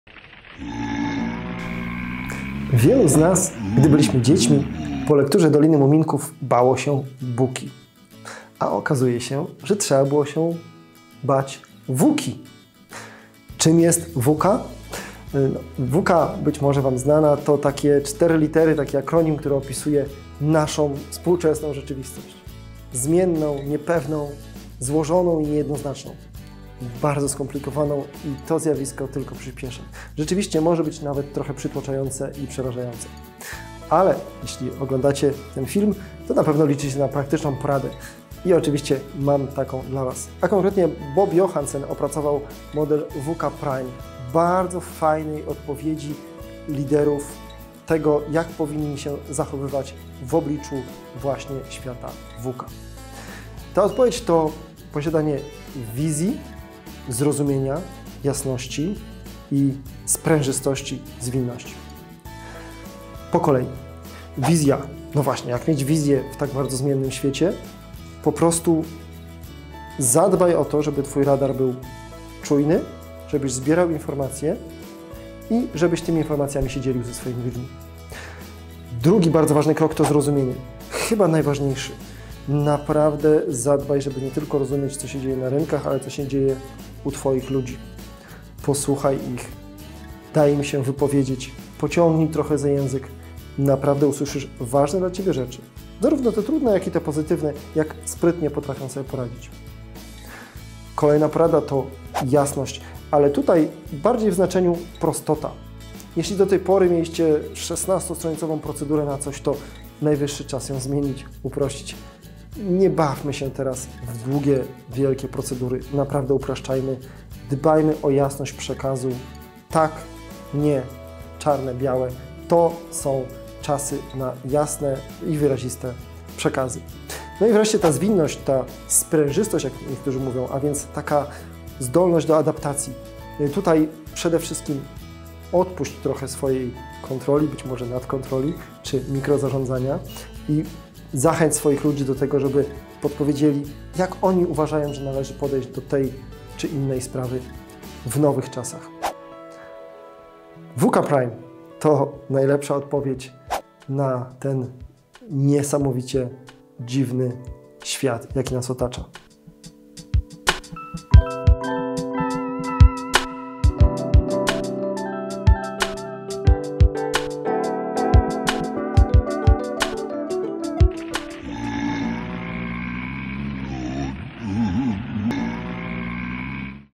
Czy należy bać się współczesnej rzeczywistości? Arkadiusz Siechowicz Vuca Prime2020
Jak mieć wizję w tak bardzo zmiennym świecie?
Czym jest VUCA?
0:00 Buka
0:13 VUCA
0:42 VUCA PRIME 2020
1:03 Wizja
1:19 Zrozumienie
1:41 Jasności
2:07 Zwinność
2:50 Żegnaj Buko
Chcesz nas lepiej poznać? zapraszamy tu:

Produkcja: Westhill Consulting Video Production Studio
Wystąpił: Arkadiusz Siechowicz - Partner Zarządzający Westhill Consulting
Zdjęcia / Montaż / Postprodukcja: Marcin Wójcik

AS Vuca prime
Wielu z nas, gdy byliśmy dziećmi, po lekturze "Doliny Muminków" bało się Buki, a okazuje się, że trzeba było się bać vuci.
Czym jest VUCA?
VUCA być może wam znana, to takie cztery litery, taki akronim, który opisuje naszą współczesną rzeczywistość, zmienną, niepewną, złożoną i niejednoznaczną, bardzo skomplikowaną i to zjawisko tylko przyspiesza.
Rzeczywiście może być nawet trochę przytłaczające i przerażające, ale jeśli oglądacie ten film, to na pewno liczycie na praktyczną poradę, ja oczywiście mam taką dla was.
A konkretnie Bob Johansen opracował model VUCA Prime, bardzo fajnej odpowiedzi liderów tego, jak powinni się zachowywać w obliczu właśnie świata VUCA.
Ta odpowiedź to posiadanie wizji, zrozumienia, jasności i sprężystości, zwinności.
Po kolei.
Wizja, no właśnie, jak mieć wizję w tak bardzo zmiennym świecie?
Po prostu zadbaj o to, żeby twój radar był czujny, żebyś zbierał informacje i żebyś tymi informacjami się dzielił ze swoimi ludźmi.
Drugi bardzo ważny krok, to zrozumienie, chyba najważniejszy.
Naprawdę zadbaj, żeby nie tylko rozumieć, co się dzieje na rynkach, ale co się dzieje u twoich ludzi.
Posłuchaj ich, daj im się wypowiedzieć, pociągnij trochę za język, naprawdę usłyszysz ważne dla ciebie rzeczy.
Zarówno te trudne, jak i te pozytywne, jak sprytnie potrafią sobie poradzić.
Kolejna porada, to jasność.
Ale tutaj bardziej w znaczeniu prostota.
Jeśli do tej pory mieliście szesnastostronicową procedurę na coś, to najwyższy czas ją zmienić, uprościć.
Nie bawmy się teraz w długie, wielkie procedury, naprawdę upraszczajmy, dbajmy o jasność przekazu: tak - nie, czarne - białe.
To są czasy na jasne i wyraziste przekazy.
No i wreszcie ta zwinność, ta sprężystość, jak niektórzy mówią, a więc taka zdolność do adaptacji.
No i tutaj przede wszystkim odpuść trochę swojej kontroli, być może nadkontroli, czy mikrozarządzania i zachęć swoich ludzi do tego, żeby podpowiedzieli, jak oni uważają, że należy podejść do tej czy innej sprawy w nowych czasach.
VUCA Prime to najlepsza odpowiedź na ten niesamowicie dziwny świat, jaki nas otacza.